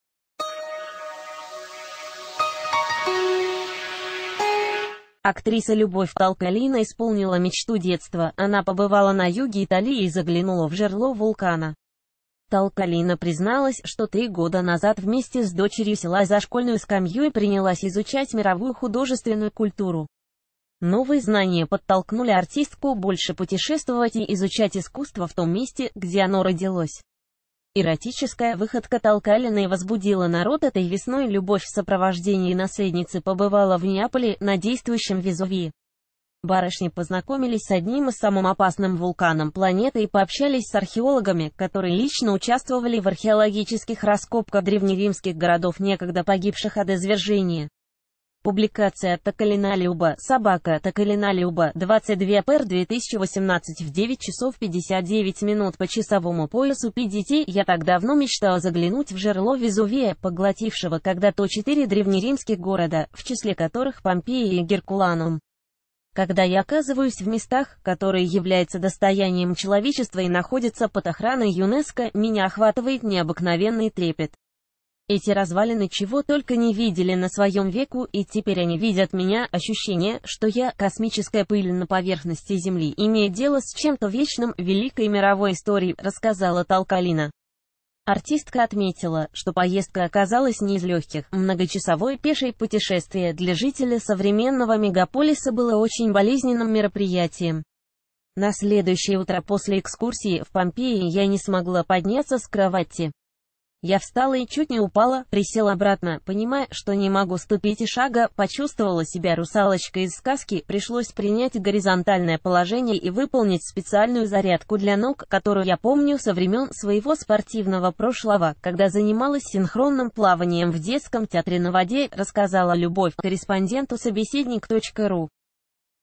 Толкалина подвергла себя страшной опасности на отдыхе | TVRu
Актриса Любовь Толкалина исполнила мечту детства. Она побывала на юге Италиии заглянула в жерло вулкана. Толкалина призналась, что три года назад вместе с дочерью села за школьную скамью и принялась изучать мировую художественную культуру. Новые знания подтолкнули артистку больше путешествовать и "и...